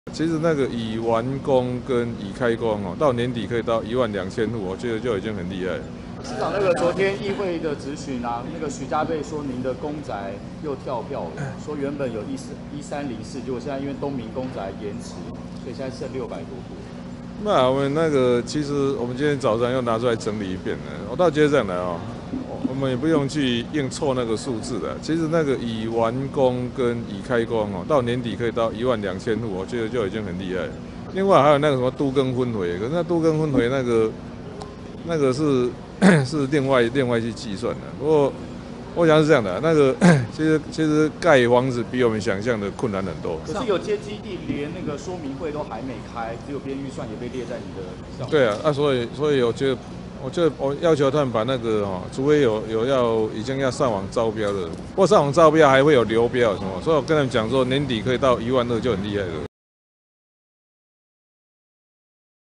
4年2萬公宅再被轟跳票 柯P：年底有1.2萬戶就很厲害了
4年2萬公宅再被轟跳票　柯P：完工、開工1.2萬戶就很厲害
☛即新聞粉絲專頁
☛合作洽談信箱 （或私訊即新聞粉絲專頁）